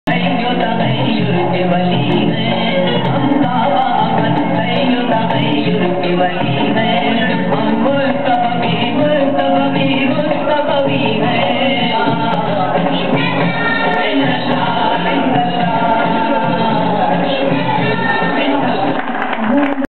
YASMIN YOUSAF
A national song of Pakistan sung by a Daughter of Pakistan on Pakistan 60th Independence day organized by Daily Perwaz, INSPAD Belgium & Friends